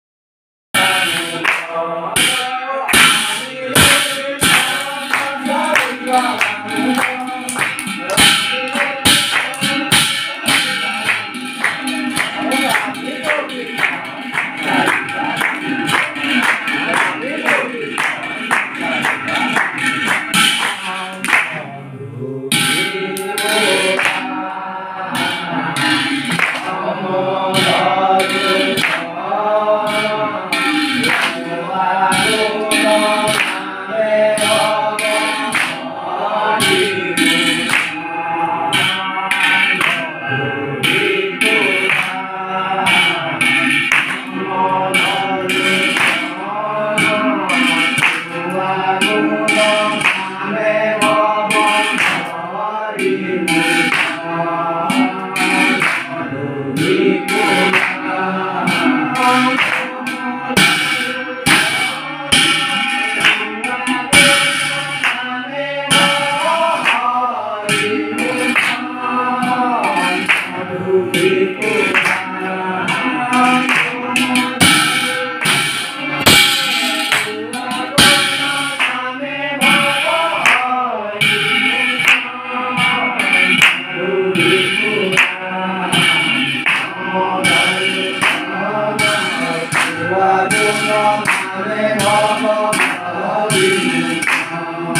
নাম প্ৰসংগ/Assamese culture of Naam Prashang for the welfare of the family//Shankar Sangh Naam
@English dm Online